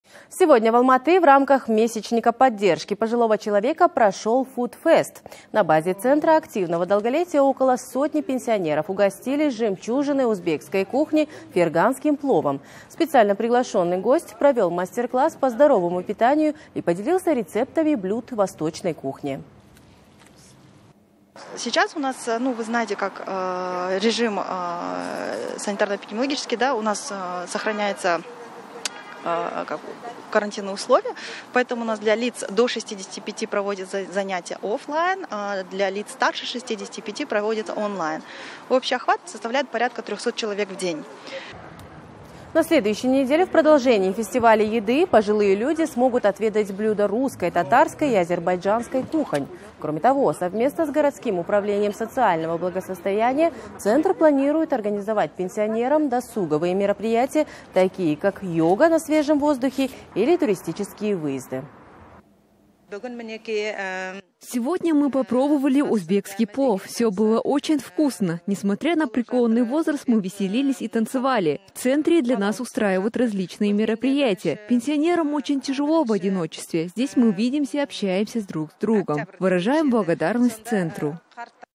В Алматы прошел фестиваль еды: какие еще мероприятия ждут пенсионеров (06.10.20)
В Алматы прошел фуд-фест в рамках месячника поддержки пожилого человека. На базе Центра активного долголетия около ста пенсионеров отведали ферганский плов. Специально приглашенный гость провел мастер-класс по здоровому питанию и поделился рецептами блюд восточной кухни, передает Almaty.tv